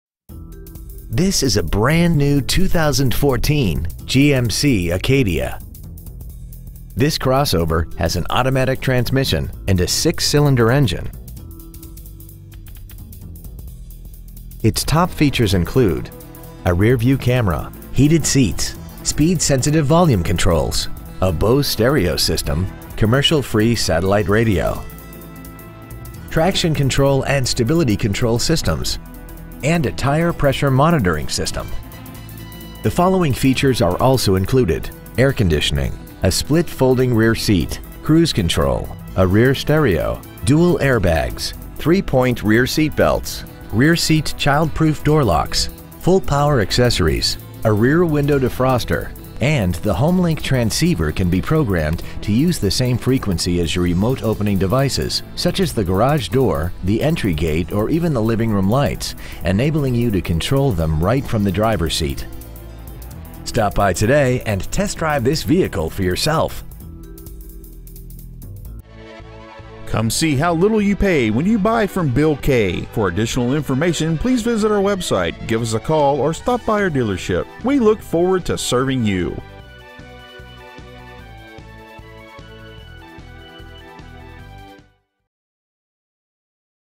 2014 GMC Acadia Downers Grove IL
We have been honored to serve the Downers Grove IL area , we promise that your experience at our dealership will exceed your expectations ! Year : 2014 Make : GMC Model : Acadia Engine : 3.6L 6 Cyl. Fuel Injected Trans . : 6-Speed Automatic Exterior : White Diamond Tricoat Stock : 148455 Bill Kay Buick GMC Ogden Avenue Downers Grove , IL 60515